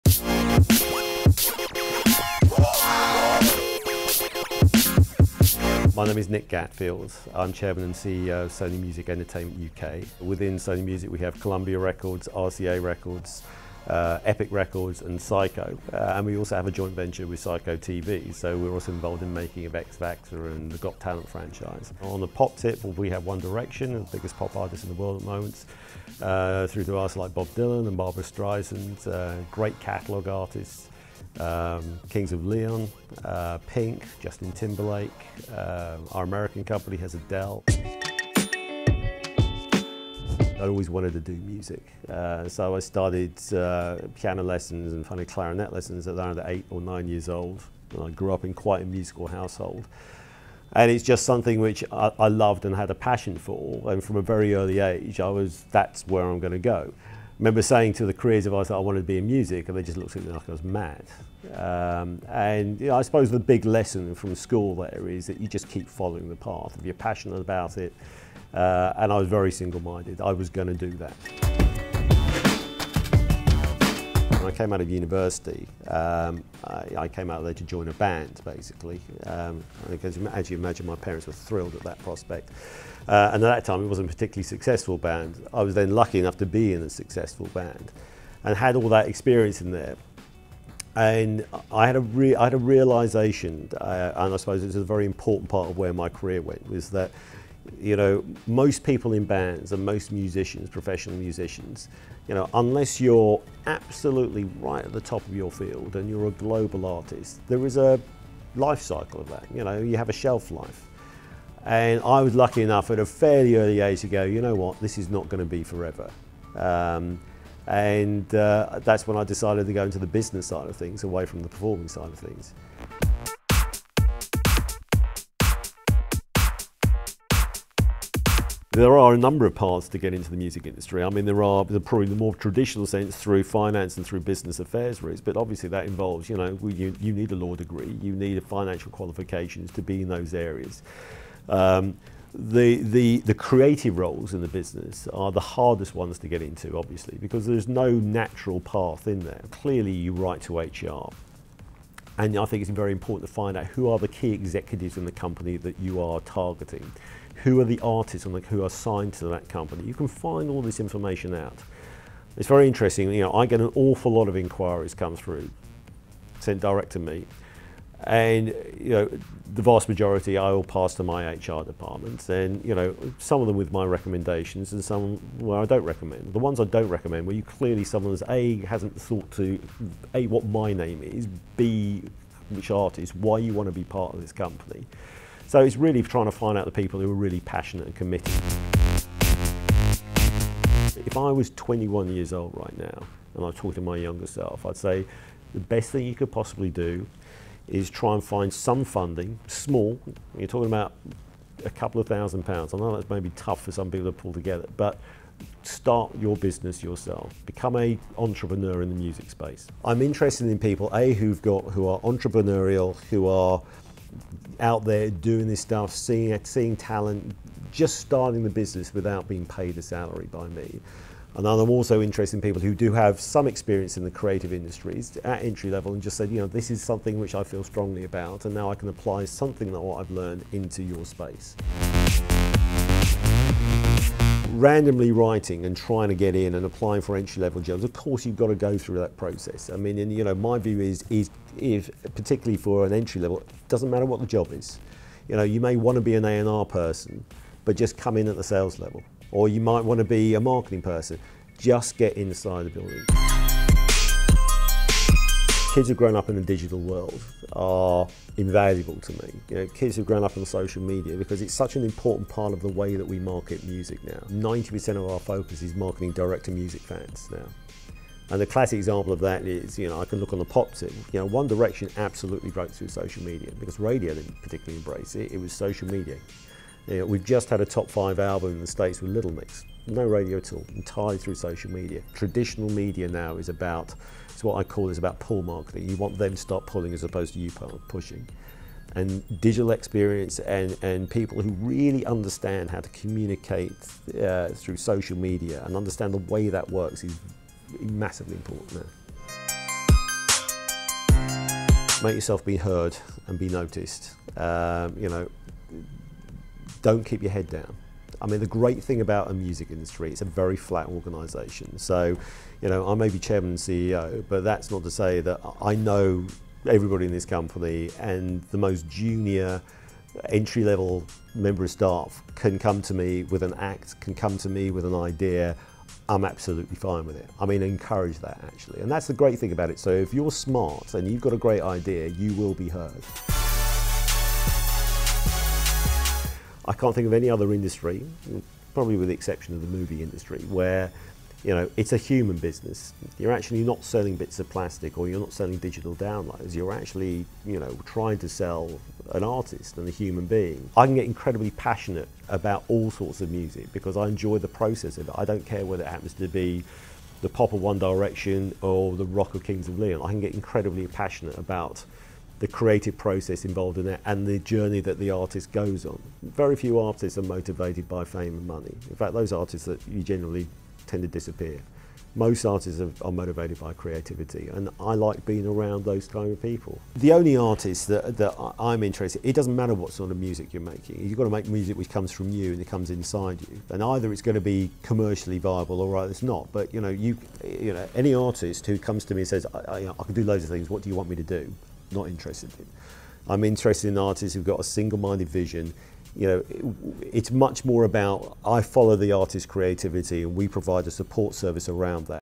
HOW TO MAKE IT - Music Industry (Top 5 Tips - Nick Gatfield, Sony Music)
TOP 5 SECRET TIPS!!!...HTMI for those that want to work in the MUSIC INDUSTRY from Nick Gatfield, CEO of Sony Music (UK). Your favourite artists are likely to come from Sony with a stable of artists as diverse as 1Direction & Justin Timberlake to Barbra Streisand & Kings Of Leon. As well as that, they team up with Simon Cowell's SYCO to make the X-Factor. So if you want to work with someone like Nick Gatfield, now's the time to start taking notes.

Nick's talks about how his involvement in music started from his passion for it as a kid and then going on to be part of Dexys Midnight Runners.  Realising that there is often a shelf life in being a musician he quickly went into the business side of the industry & found other career options.  Also keep an eye out for how & why One Direction became the juggernaut they are & advice for aspiring musicians on what he looks for when signing acts.

A MUST SEE for those that want to work in the music industry and want to get ahead.  He's really got some wise words for those that want to make it.